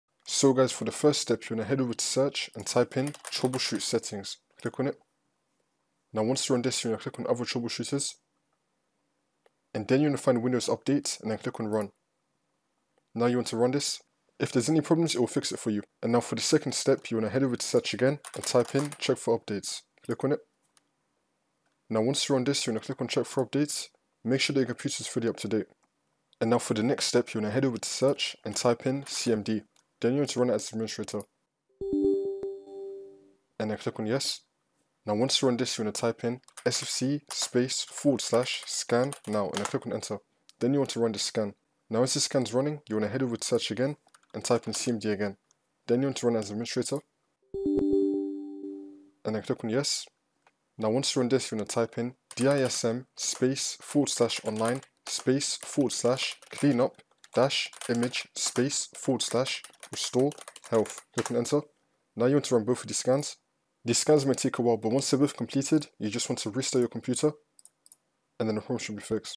How To Fix Your Device is Missing Important Security and Quality Fixes
FIX Your Device is Missing Important Security and Quality Fixes In Windows 11 & Windows 10 [Tutorial]

Thanks to Microsoft, Windows 11 users get updates on a regular basis. However, some users are seeing an error message on their update page. The error message says “Your device is missing important security and quality fixes”. Usually, this type of error message will prove to be a very useful reminder to install important updates but the issue here is that users are seeing this message even after installing all the updates. In tutorial, you will keep seeing an error message telling you to install important updates even if there aren’t any updates available to install.

On a Windows 11 based computer, the following warning message is displayed in Update and Security center: "We couldn't connect to the update service. Your device is at risk because it's out of date and missing important security and quality updates. Let’s get you back on track so Windows can run more securely."

This tutorial will apply for computers, laptops, desktops,and tablets running the Windows 11 & Windows 10 operating system (Professional, Enterprise, Education) from all supported hardware manufactures, like Dell, HP, Acer, Asus, Toshiba,Lenovo, and Samsung).